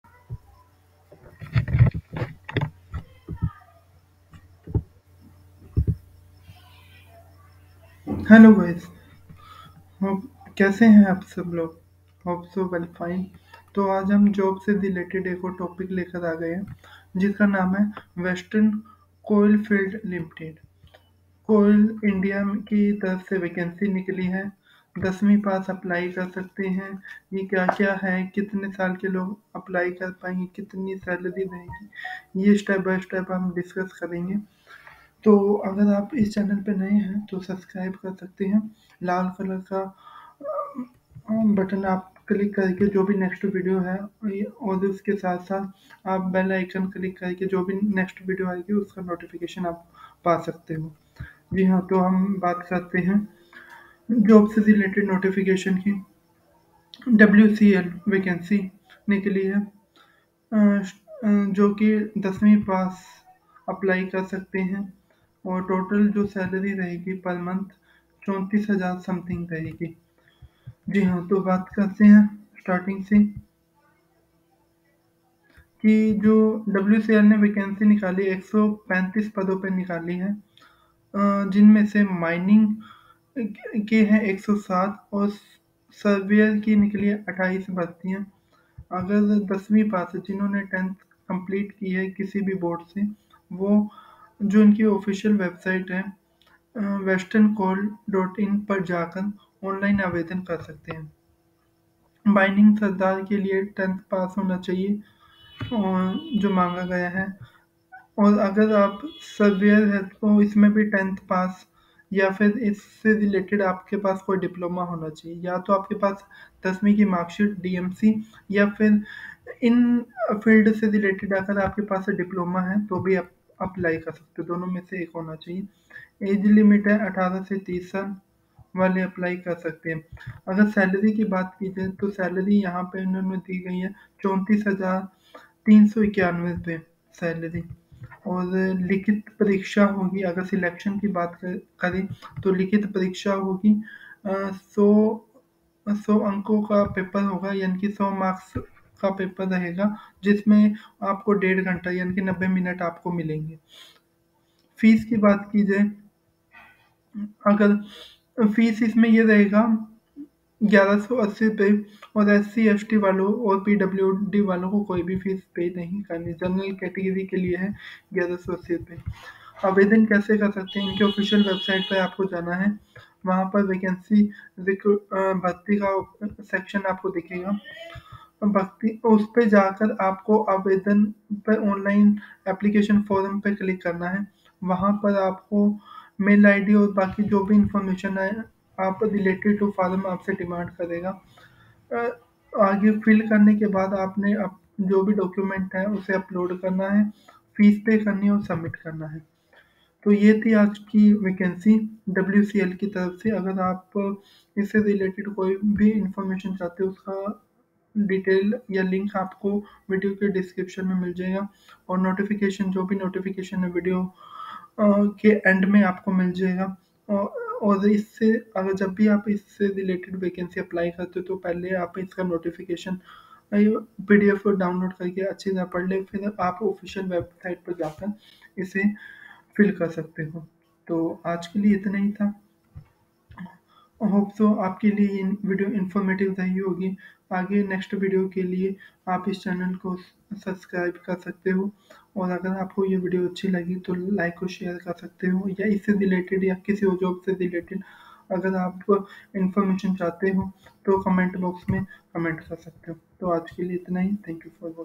WCL Vacancy 2023-24,Online Apply Now
10वीं पास डब्ल्यूसीएल में वैकेंसी:30 साल तक के कर सकेंगे अप्लाई, 34,391 तक सैलरी (Vacancy in 10th pass WCL: up to 30 years will be able to apply, salary up to 34,391)